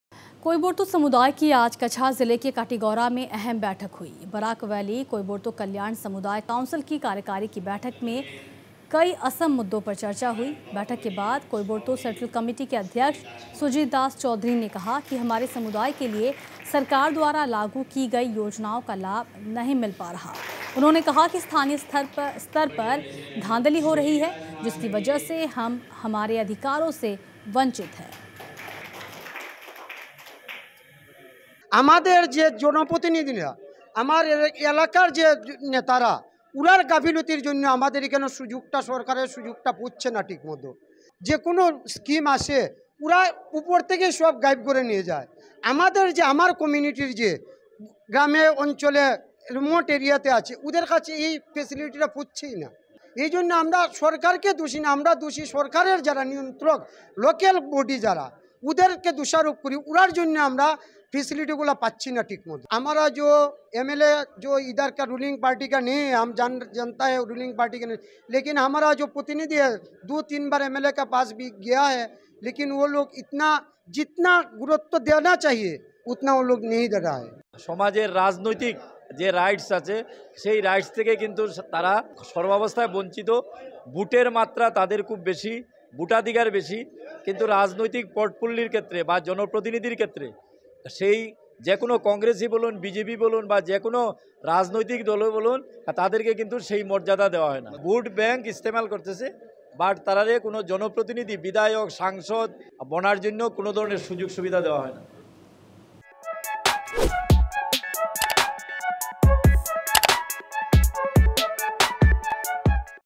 कोईबोर्तो समुदाय के लोगों को नहीं मिल रहा है योजनाओं का लाभ
कोईबोर्तों समुदाय की आज कछार जिले के काटीगोरा में अहम बैठक हुई। बराक वैली कोईबोर्तो कल्याण समुदाय काउंसिल की कार्यकारी की बैठक में कई असम मुद्दों पर चर्चा हुई।